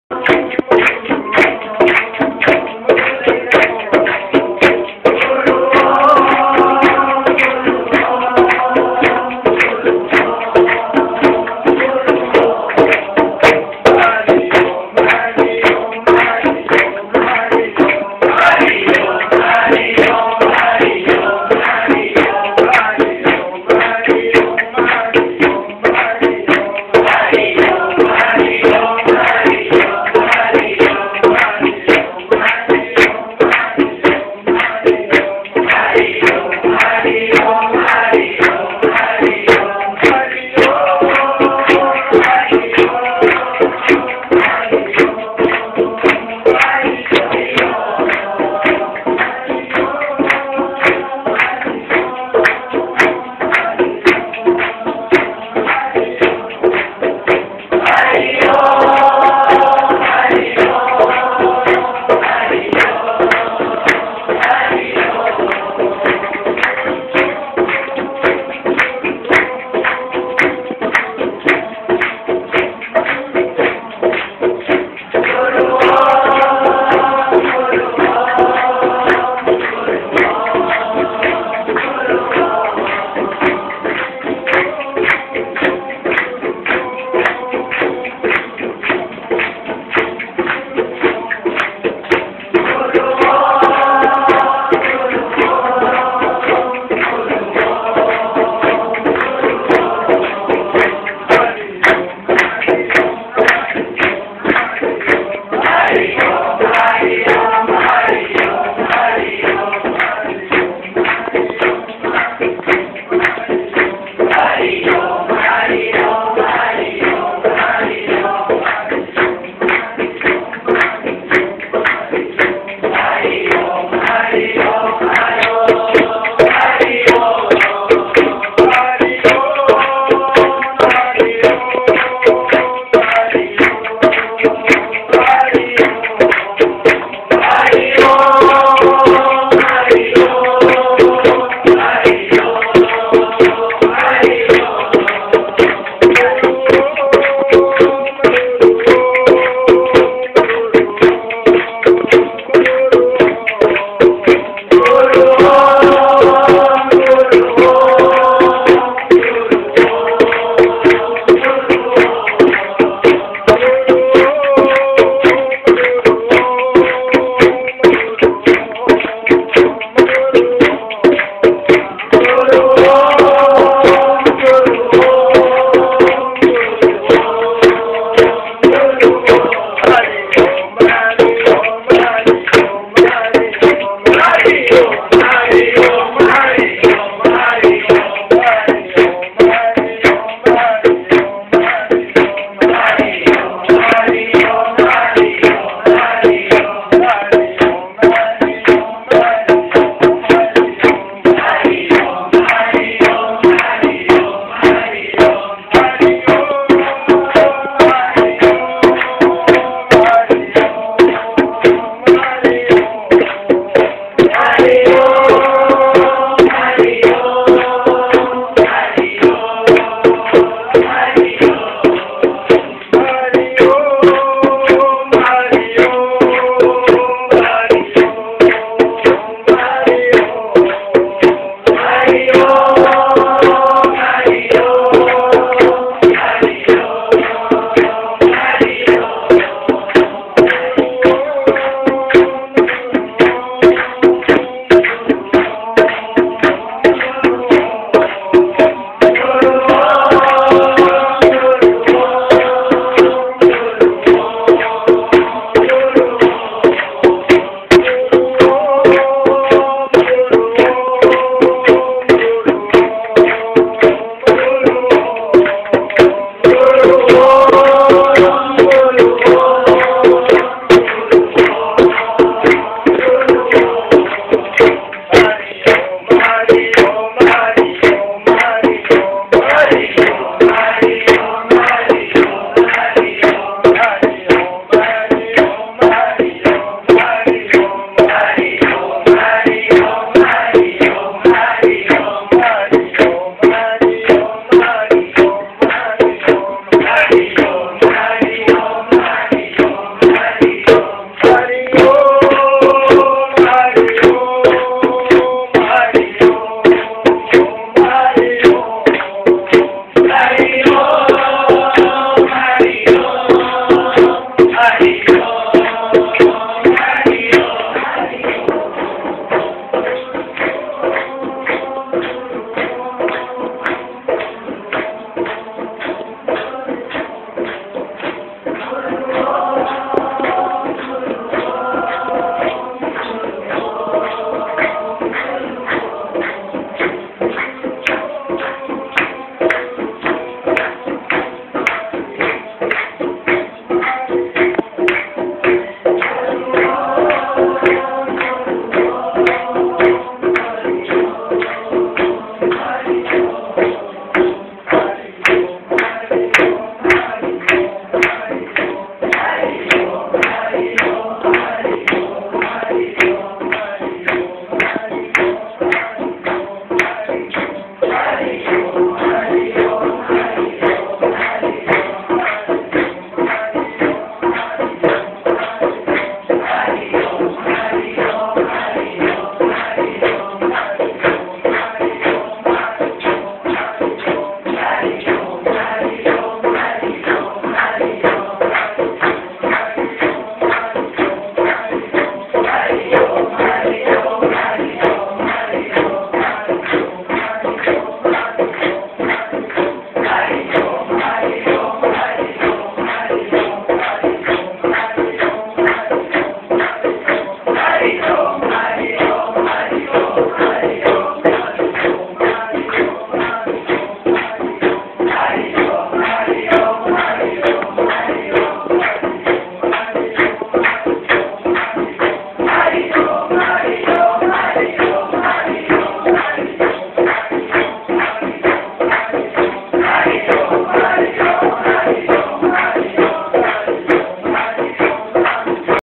Hari:Om Dhun-Gurupurnima 2014, Pujya shree Mota Part-2
On 12/7/2014 Gurupurnima Dhun at Surat, Hari:Om Ashram. Pujya shree Mota.